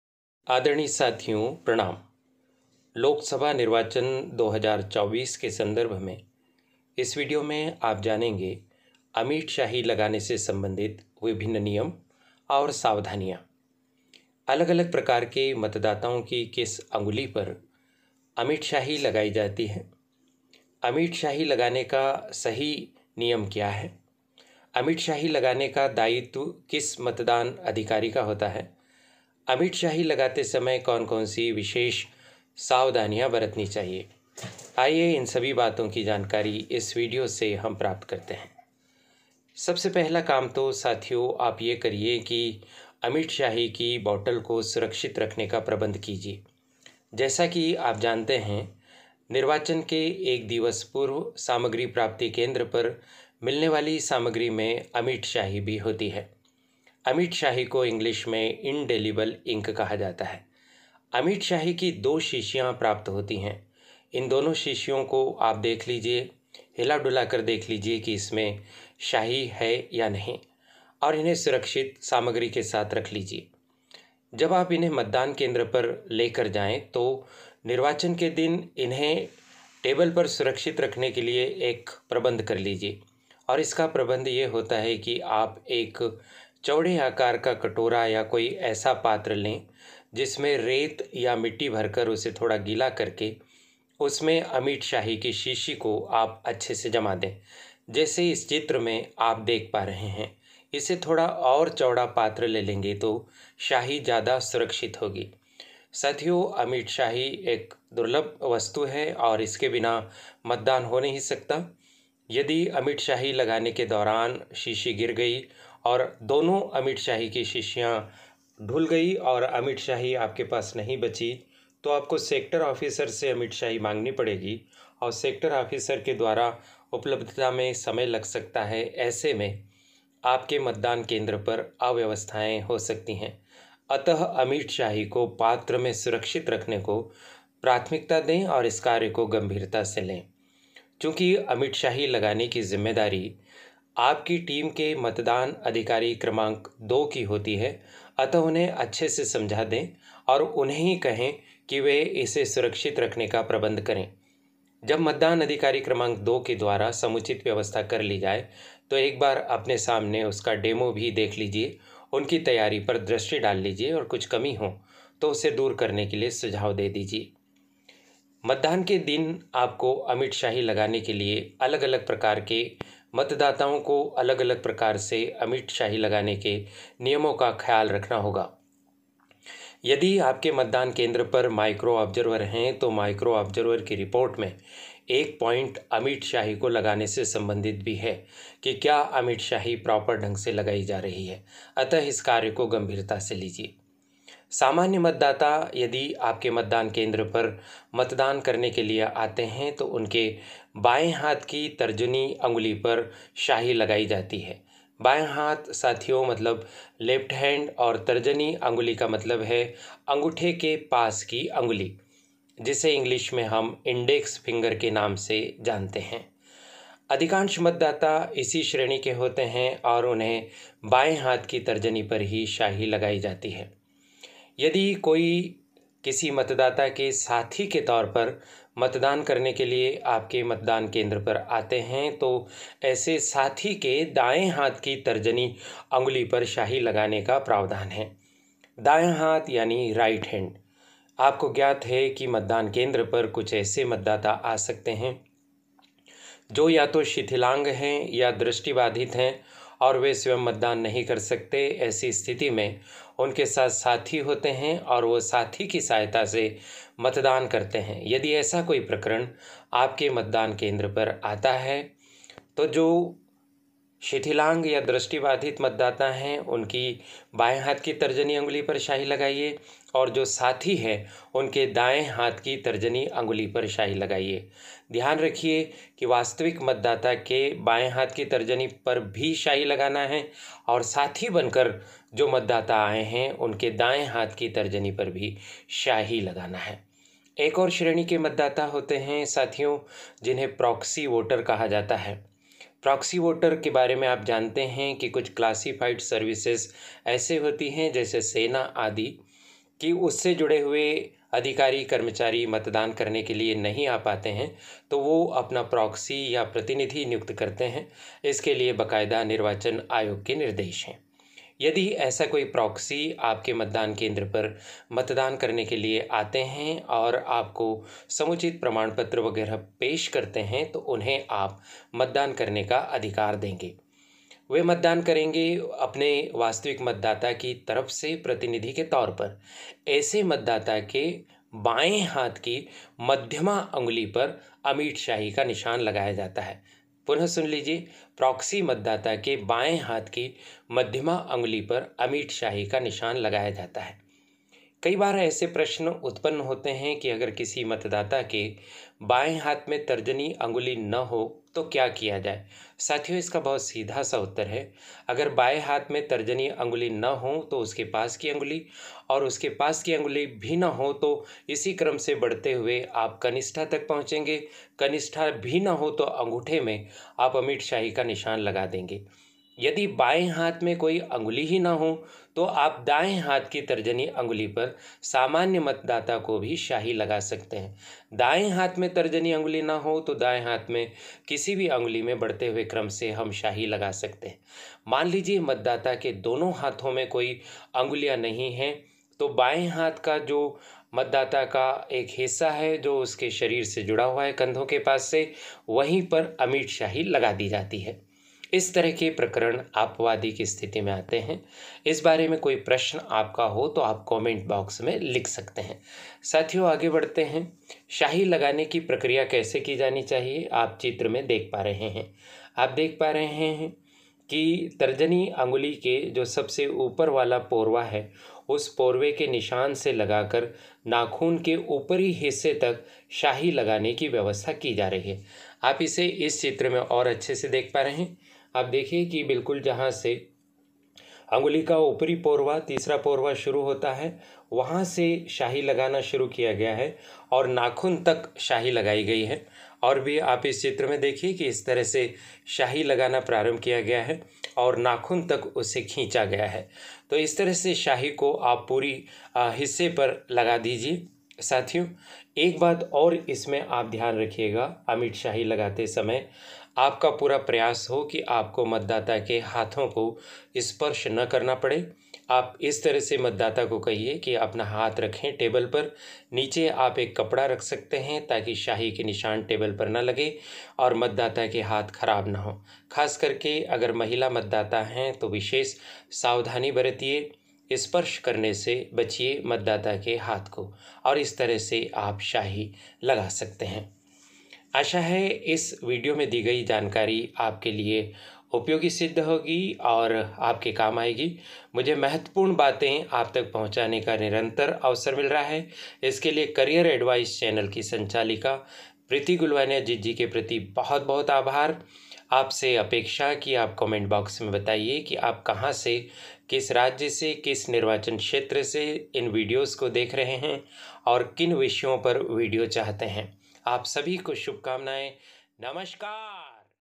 अमिट स्याही लगाने के नियम
यदि आपकी ड्यूटी आगामी लोकसभा  चुनाव को सम्पन्न  करने के लिए पीठासीन अधिकारी या मतदान अधिकारी या सेक्टर ऑफिसर या माइक्रो ऑब्जर्वर या मास्टर ट्रेनर आदि के रूप में लगी है तो यह वीडियो आपके लिए बहुत उपयोगी सिद्ध होगा...
वीडियो देखने के बाद यह अवश्य ही बताइये कि आपको इसमें दी गई जानकारी कैसी लगी?